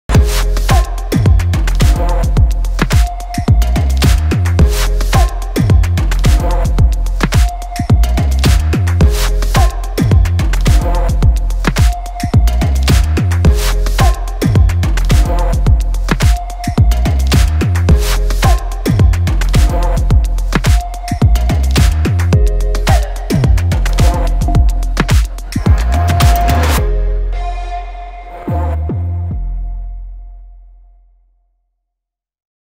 Mosogató - Koppenhága 620 Compact. Primagran HU
Mosogató - Koppenhága 620 Compact egymedencés gránit mosogatótálca csepegtetővel. Primagran